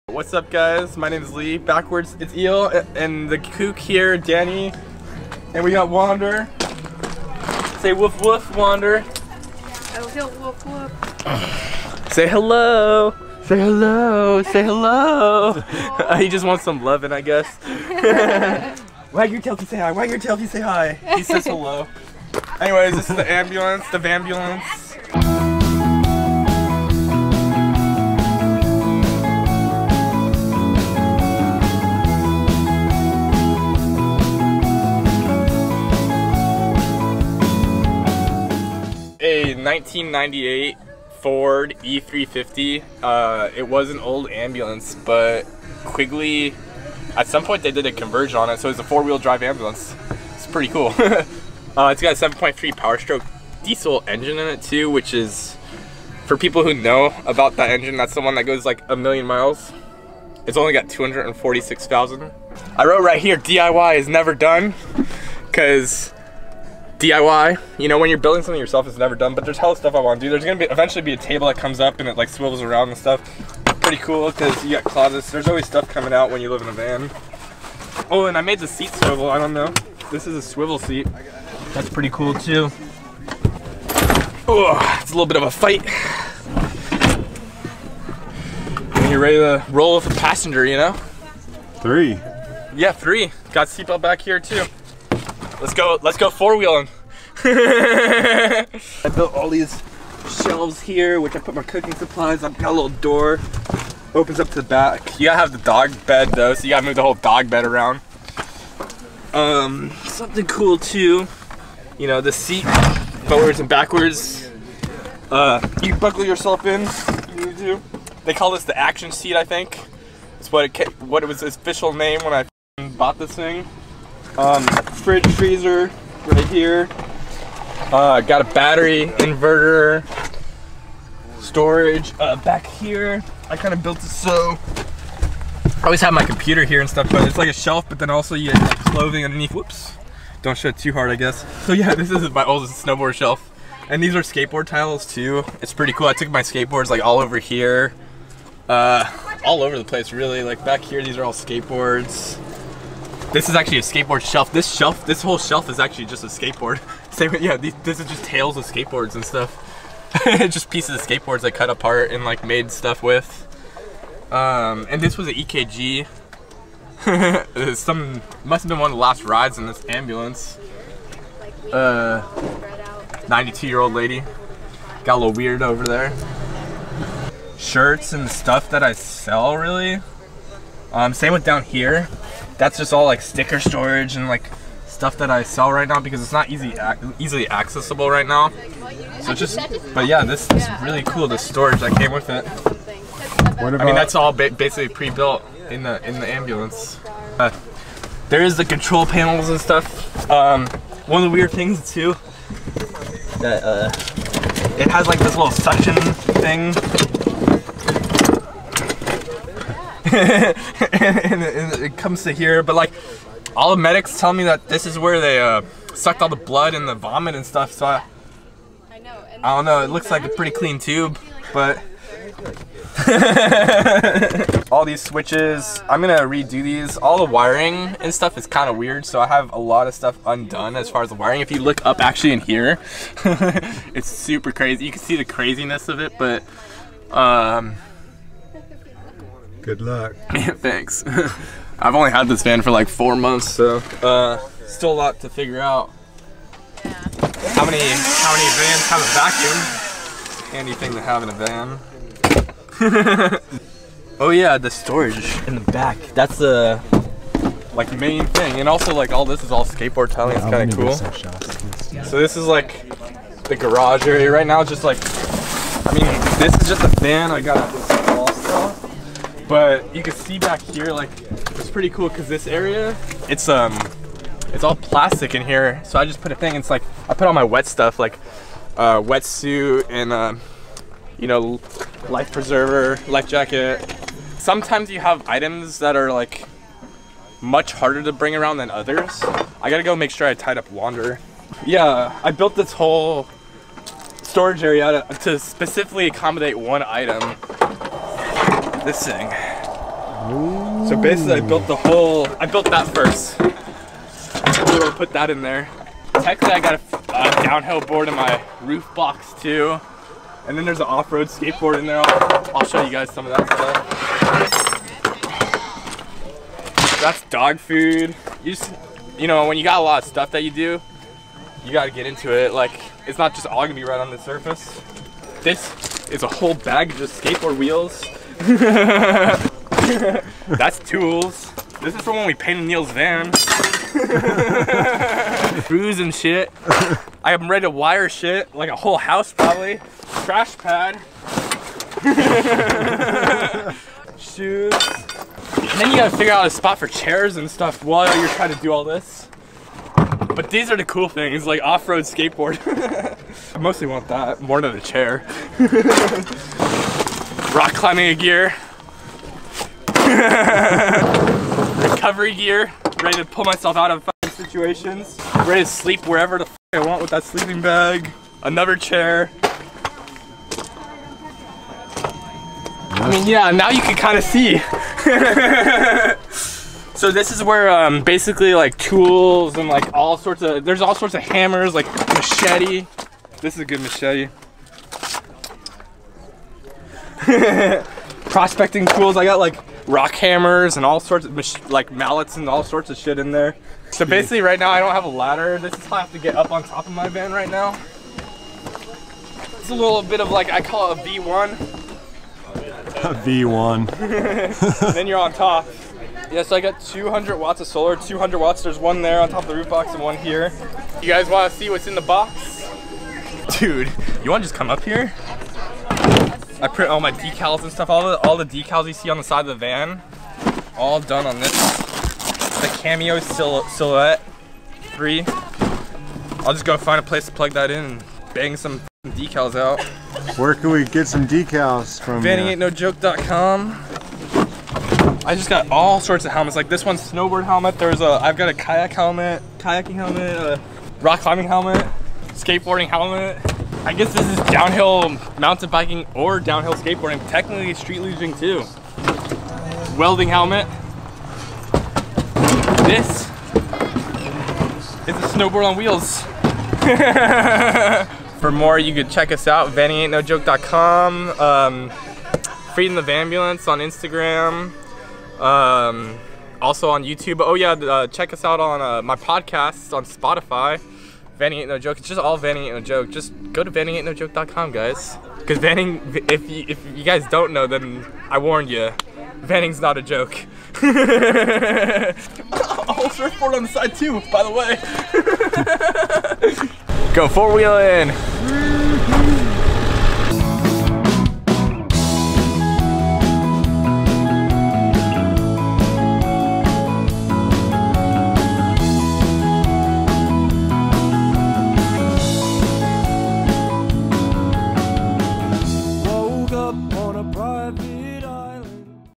Ultimate 4x4 Ambulance Campervan Conversion - Ford E350 Off Grid Ambulance Van
Lee is the ultimate vanlifer!  If you haven’t met him already and can’t tell from the video he is a fun guy to be around.  To live an adrenaline fueled life you need an adrenaline fueled van.  That’s why Lee lives in his converted four wheel drive ambulance van to go anywhere he wants.  We met Lee at Descend on Bend when he pulled us out of the sand.  Now he is back at it yanking people out of sand and trying his hardest to get his rig stuck.  Support Lee and the Kookz along the road by checking out his online magazine also check out the hilarious podcast on Spotify as well.

Check out a few items Lee had in his van
Maxx Air Fan
Goal Zero Battery
Solar Panels
Yakima Locking Roof Box